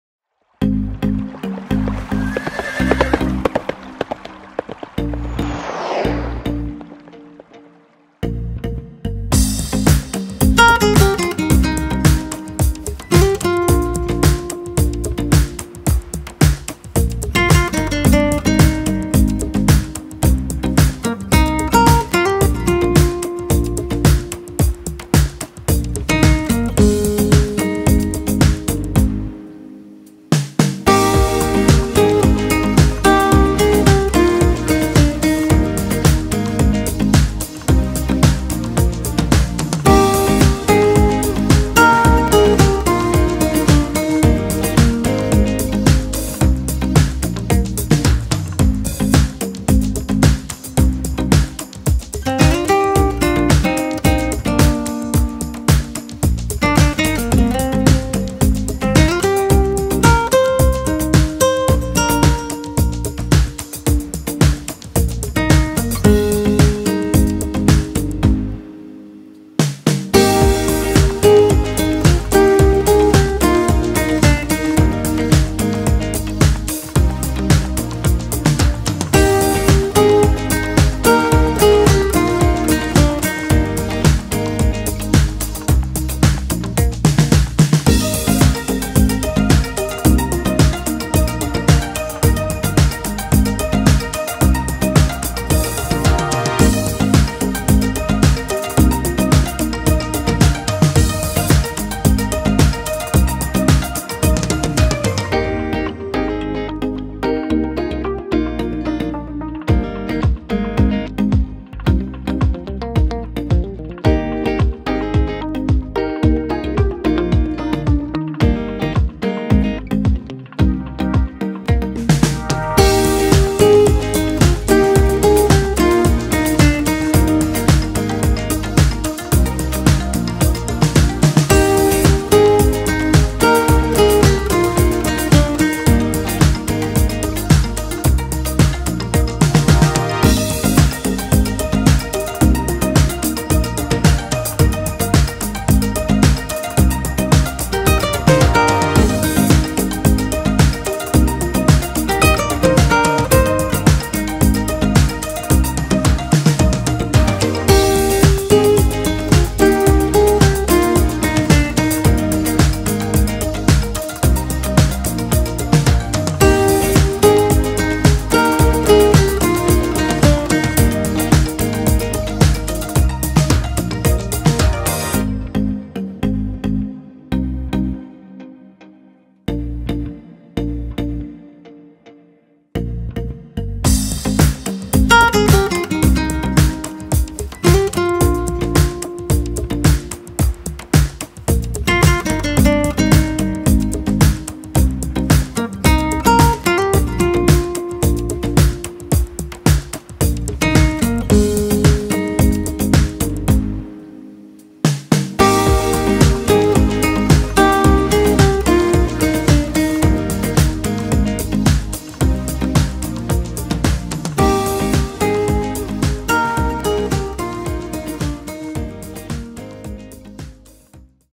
285D Michelle Vonderhaar on Piper SR Beginner Novice Dressage Twin Rivers Ranch March 2020
285D Michelle Vonderhaar on Piper SR Beginner Novice Dressage Twin Rivers Ranch March 2020